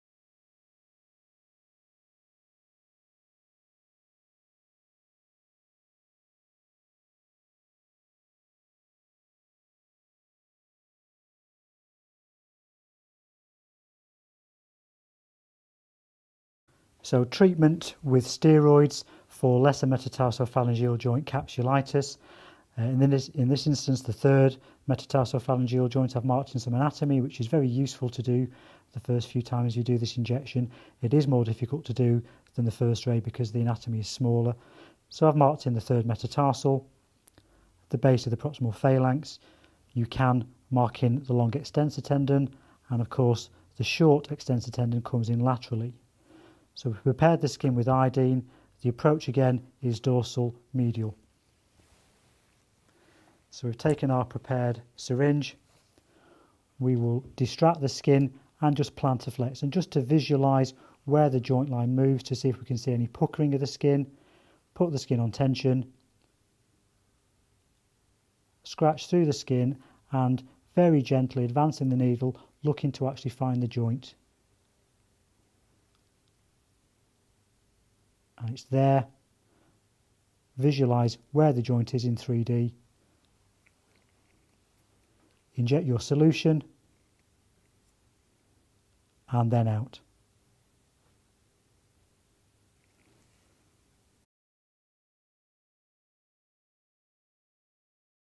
Injection - lesser MTP joint steroid injection
Injection demo (no actual steroid injectate given) taken from our 2010 techniques book of the lesser MTP joint.  This works well for capsulitis, OA and Freiberg type presentations.

Foot and Ankle Injection Techniques: Metcalf and Reilly.